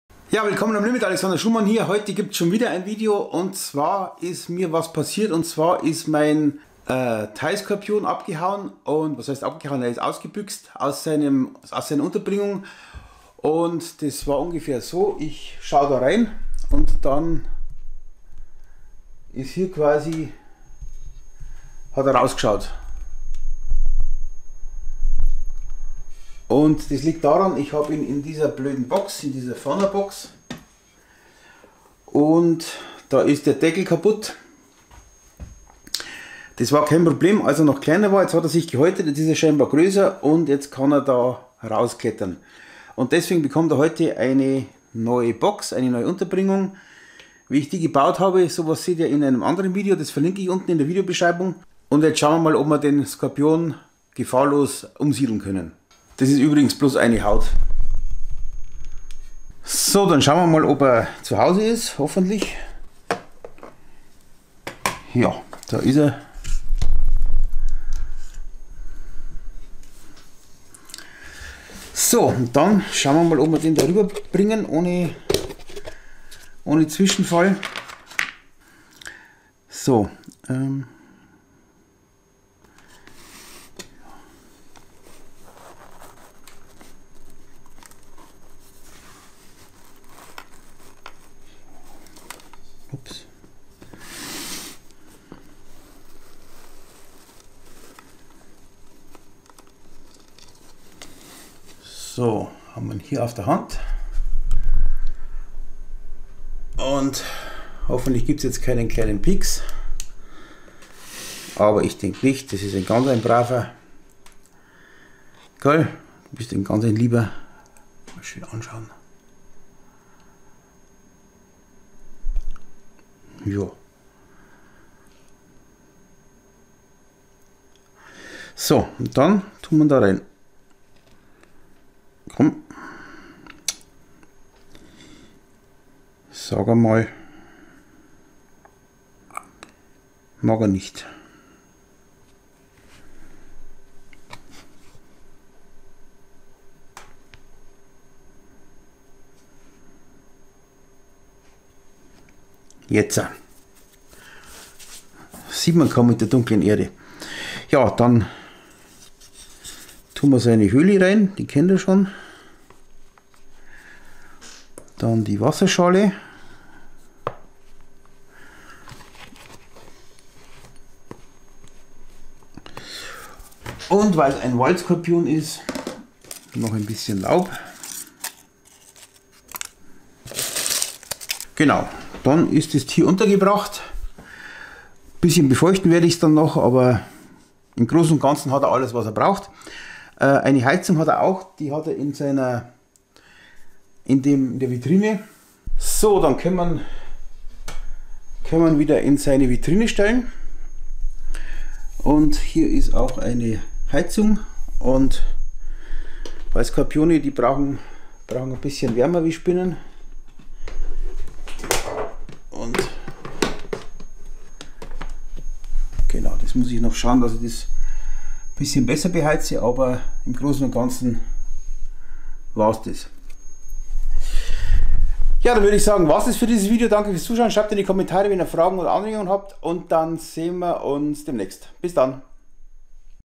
Skorpion ist ausgebüchst und muß jetzt umziehen Heterometus laoticus
Mein Thai Skorpion hat sich neulich gehäutet, und scheinbar kann er jetzt die Tür der Fauna Box öffnen. Aus diesem Grund kommt er in ein neues Terrarium, wie ich dieses gebaut habe, erkläre ich hier: